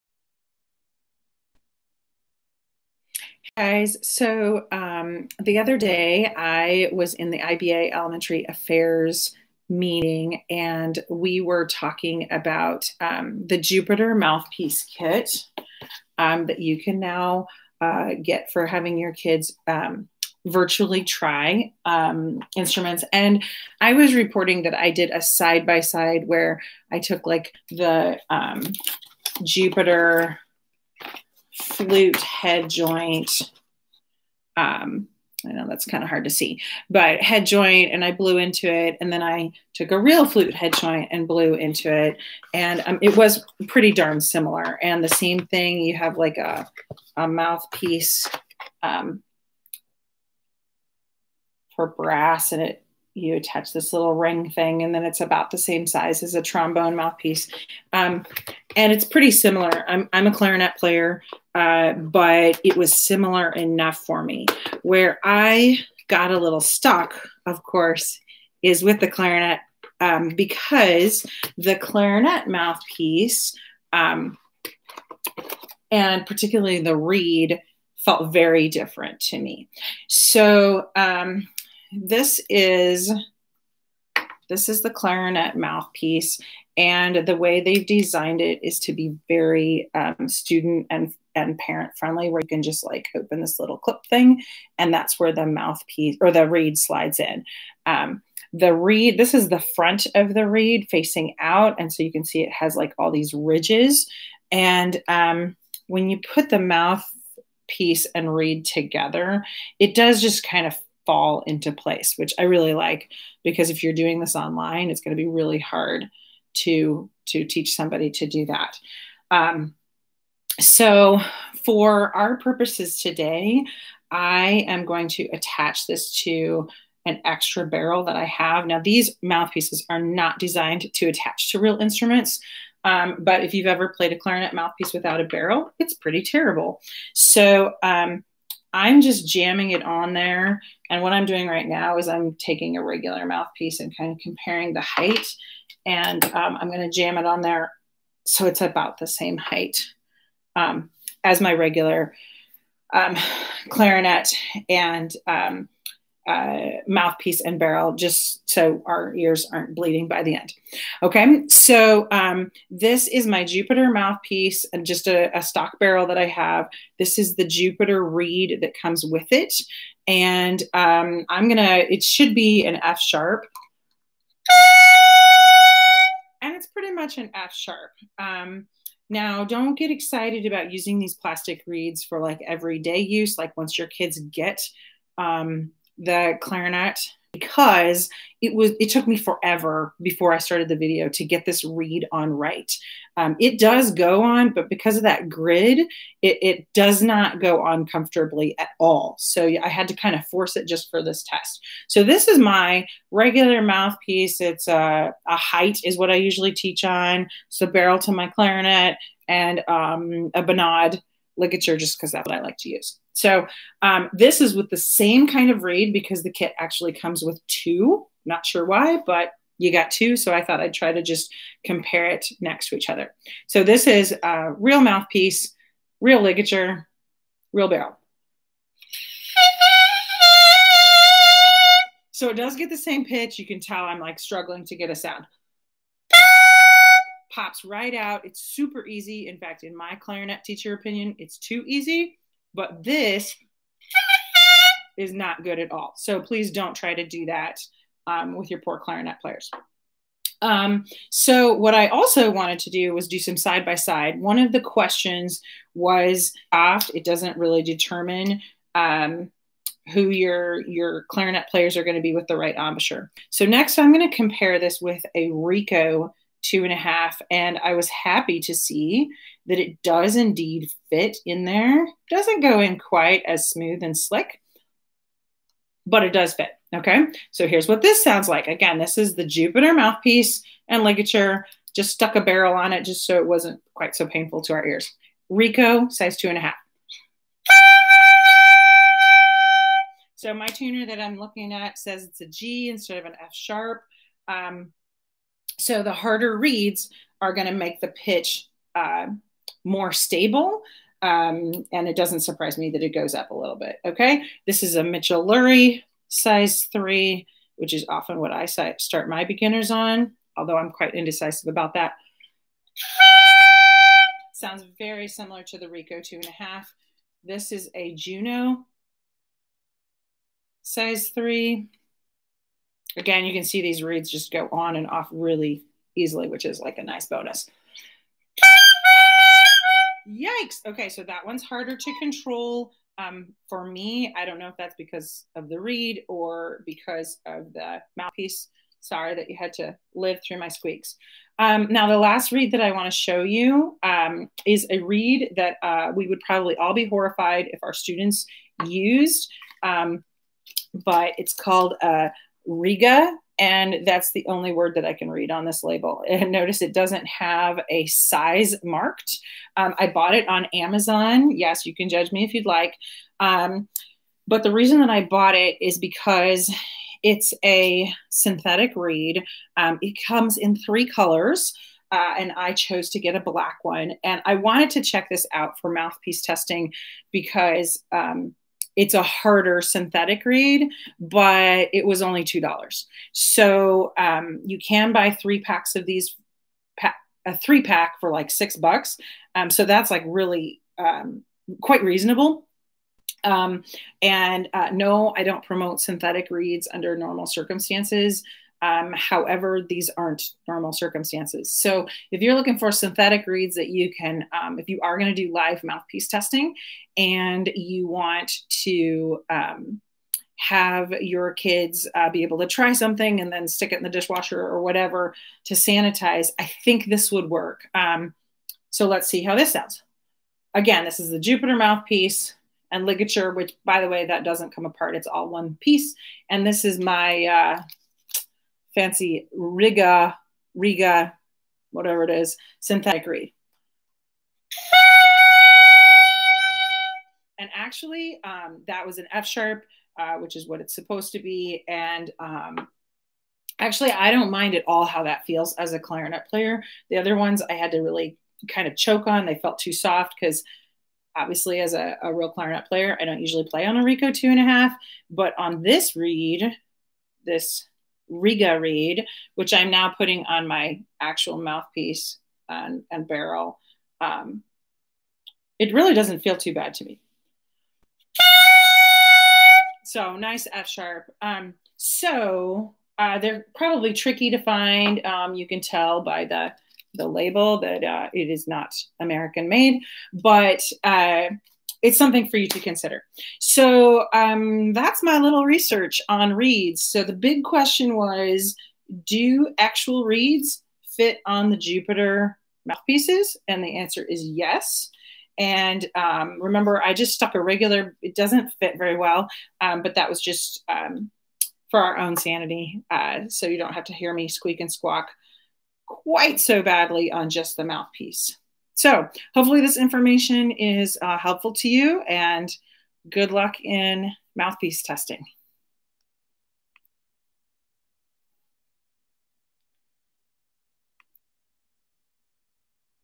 Jupiter mouthpiece kit- clarinet comparison
In this video, I compare the Jupiter Try-Out Kit clarinet mouthpiece to a regular mouthpiece, and try out different reeds.